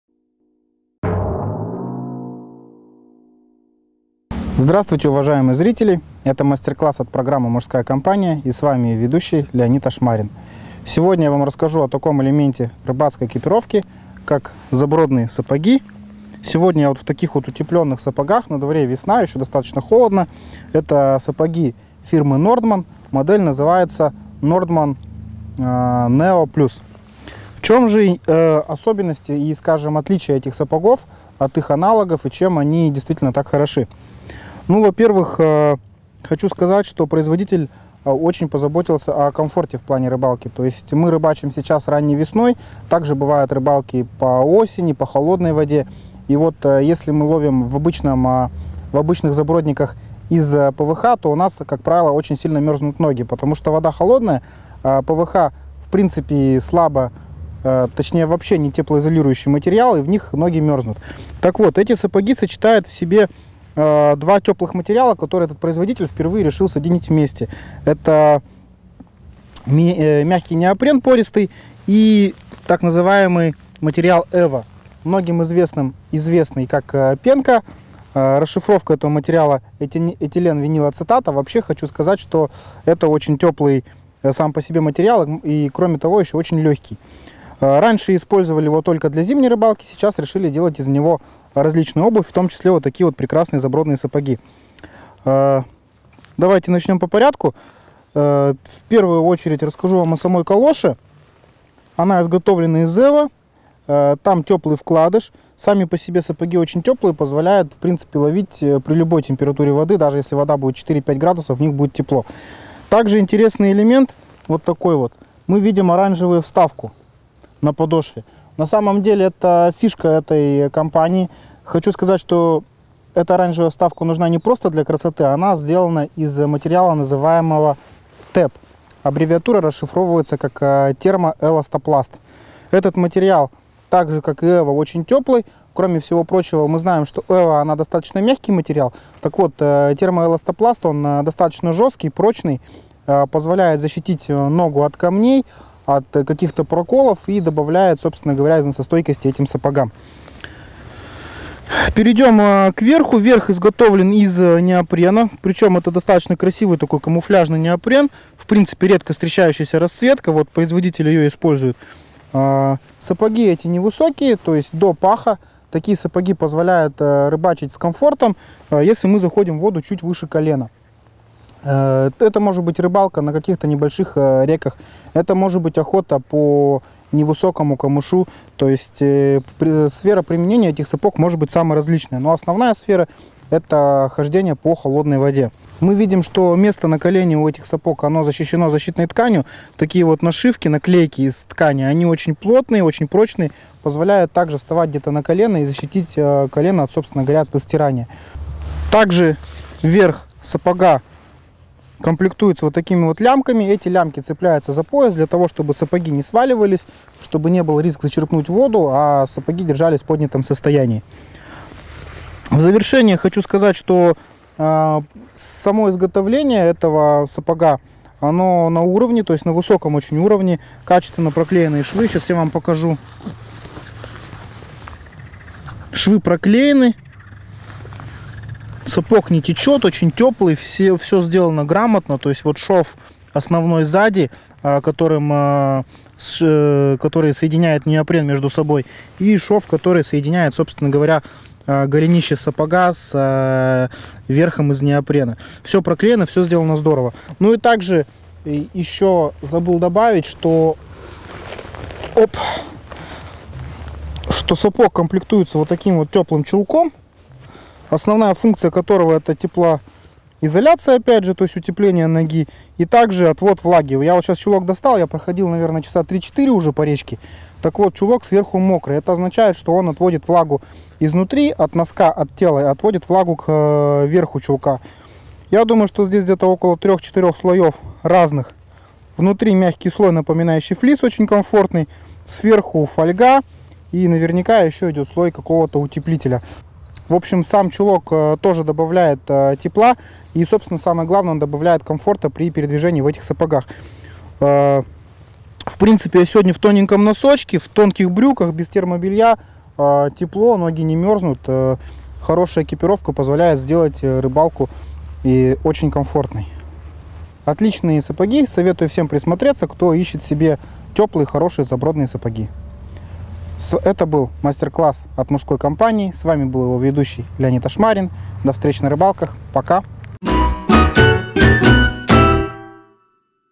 Сапоги забродные Nordman Neo Plus
Сапоги забродные Nordman Neo Plus для рыбалки в холодной воде в весенне-осенний период. Верх выполнен из трехслойного неопрена (трикотажная ткань+вспененный каучук+трикотажная ткань). Неопрен - легкий и эластичный материал с высокой теплоизоляцией.